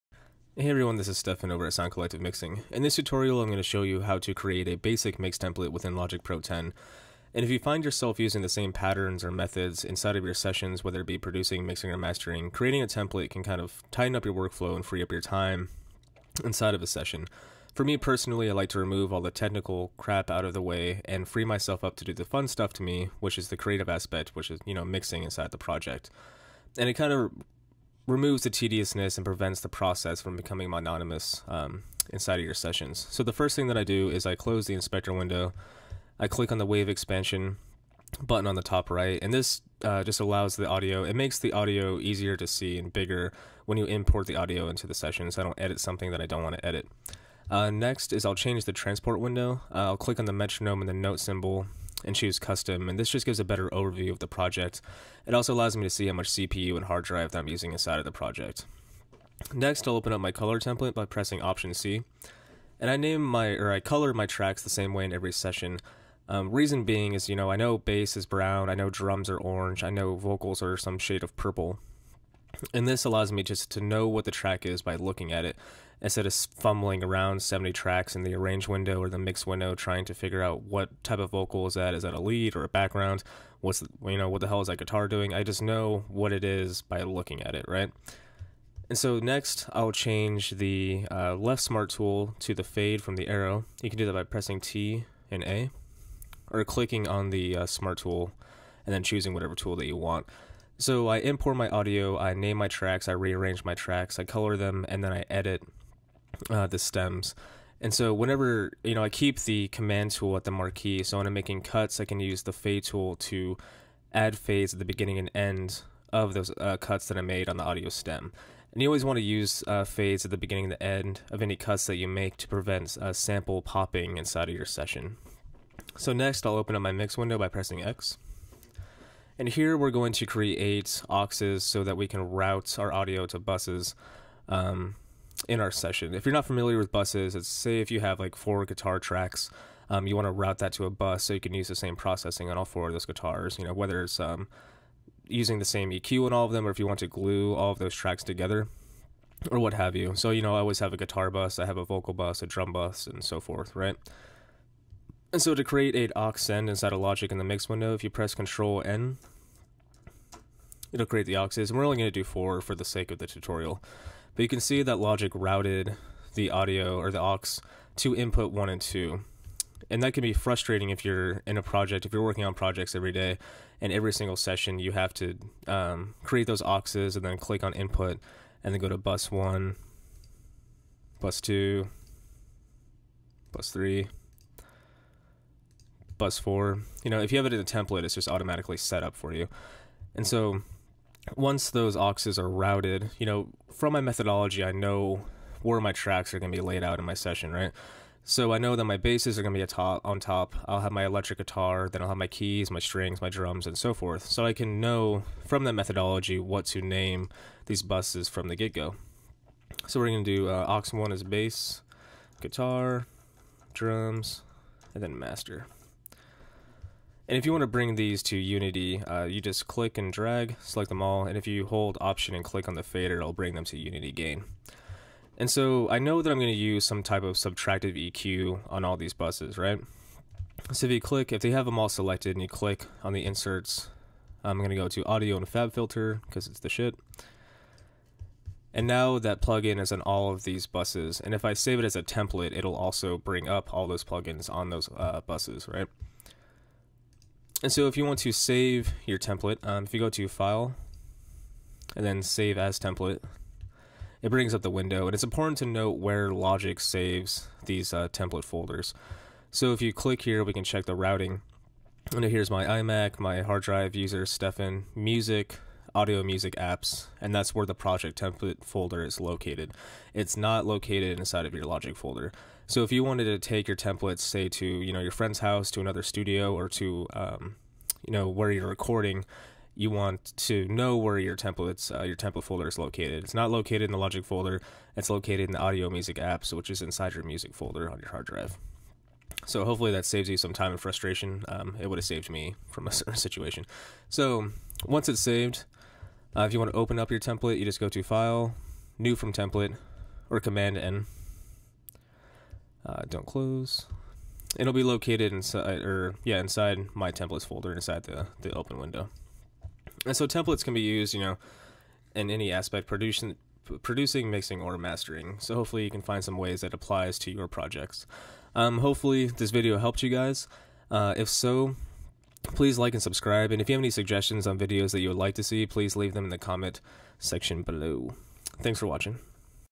Logic Pro X Template
A Logic Pro X Tutorial showing how to make a basic mix template within Logic Pro X.

Option+c: Color Palette
t: Smart Tool
x: Mix window
Option+Click on fade to bring to unity